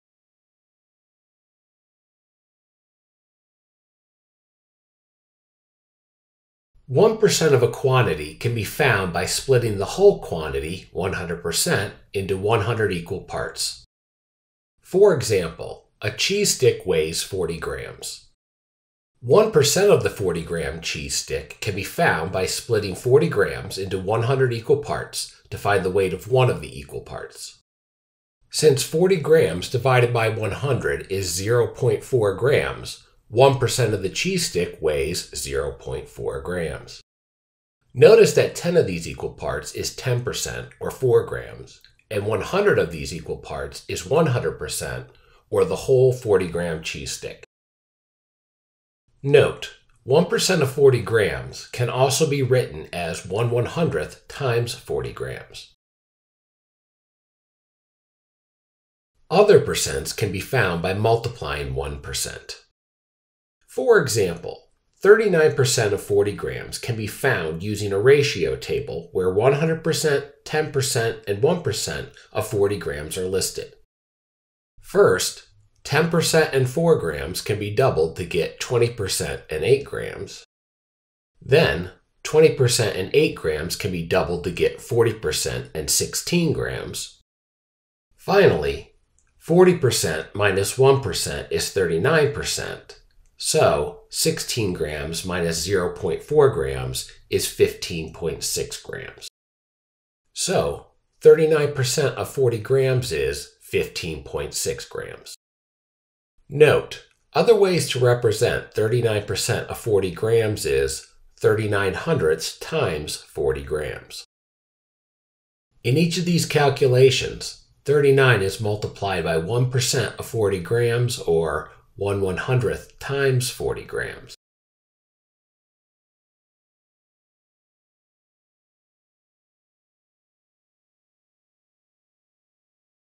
MobiusMath EMC Video - Find the Percent of a Quantity
Learn to calculate 1% and 10% of a quantity to calculate percents.